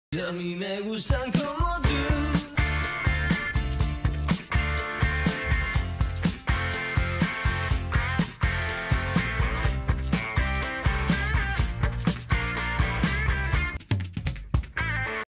SPEAK SPANISH en el Lado B de Claro música
Toda la música en español en la franja Speak Spanish para que cantes a viva voz y te dejes llevar por los sonidos de tu tierra. ¡Deja que la música te salve!